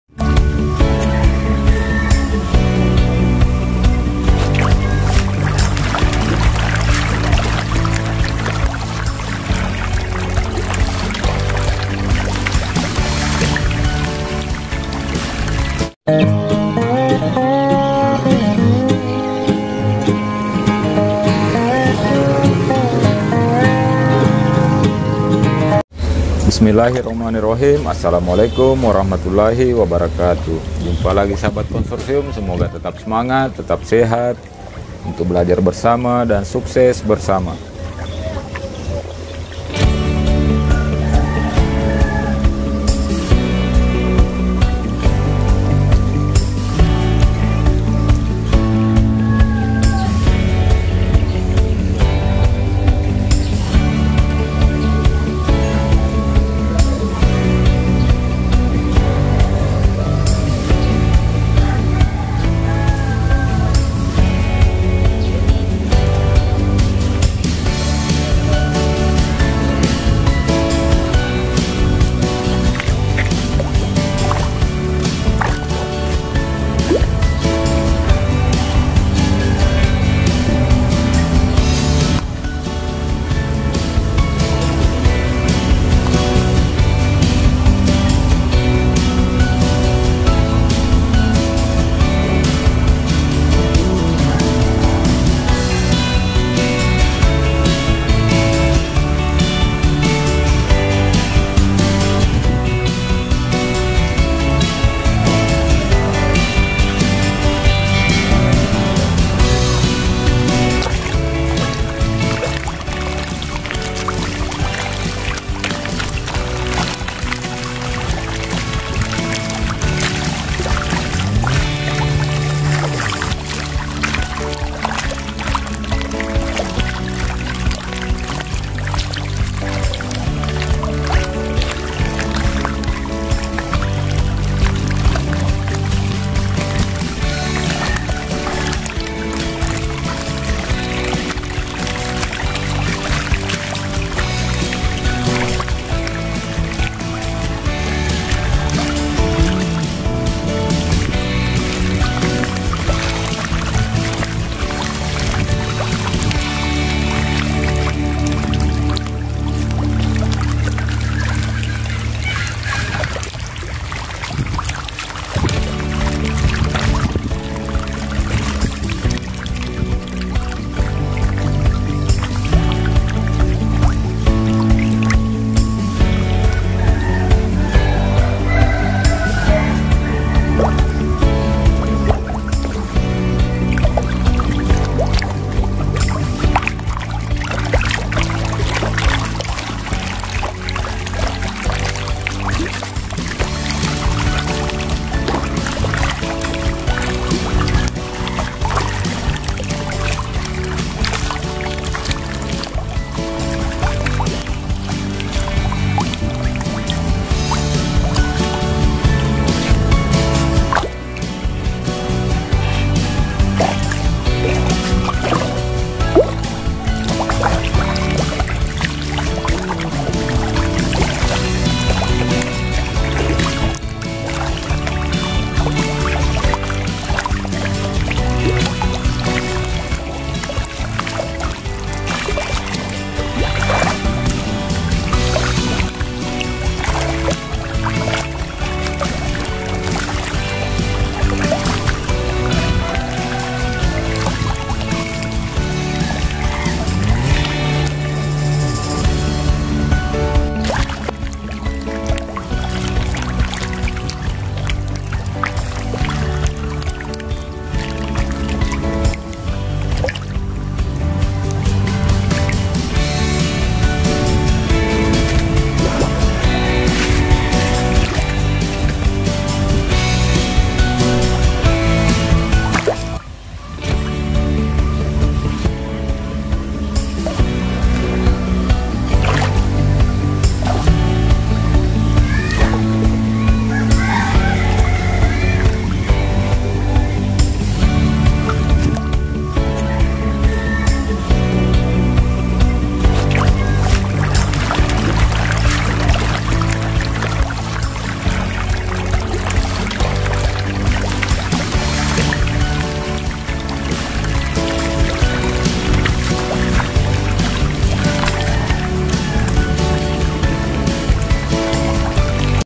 Sistem RAS berbeda, Air Bioflok terdapat Nutrisi Alami Flok || Protein Flok Capai 30-42%
Kelebihan budidaya ikan nila sistem bioflok ada pada FCR yang rendah dibanding sistem lain. Khususnya pada budidaya sistem RAS, dimana sama-sama menggunakan wadah yang sama dan air yang terbatas. Protein flok pada sistem bioflok mencapai 30-42% yang bisa menekan kebutuhan pelet

Tetap semangat tetap sehat untuk belajar bersama dan sukses bersama

sabarezfibonacci